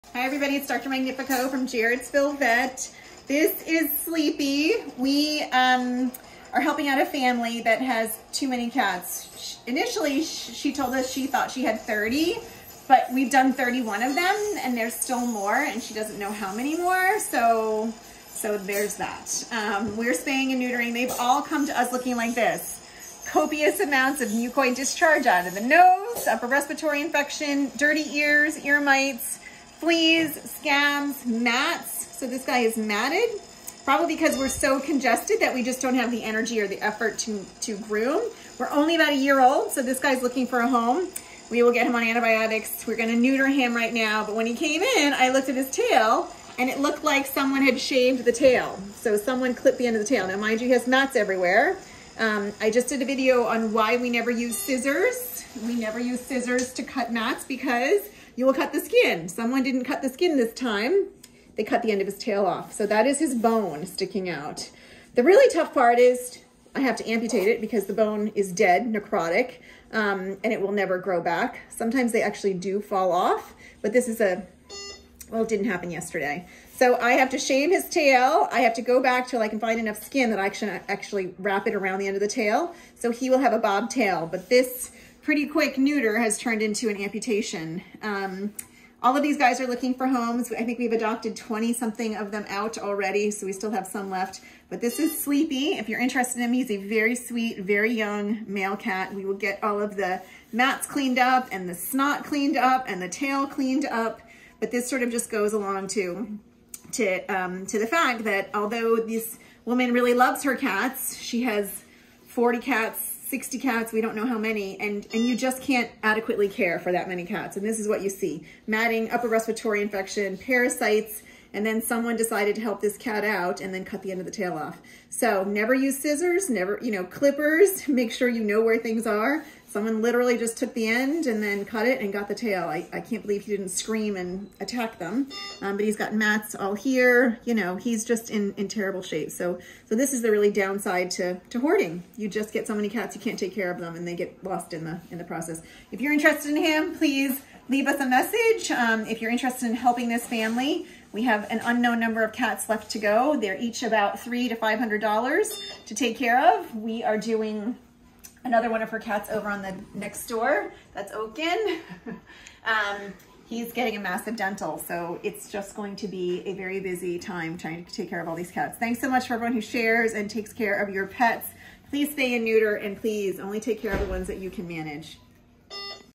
Meet Sleepy. His tail was cut too short and the top was cut off. Bone exposure occurred.
This is Sleepy. He came from a hoarding situation that my veterinary clinic is helping out with. He was found with the end of his tail cut off. How do I treat these injuries? I amputate to cover the exposed bone. I am a small animal veterinarian who posts cases I see. For free pet help find me at Pawbly.com.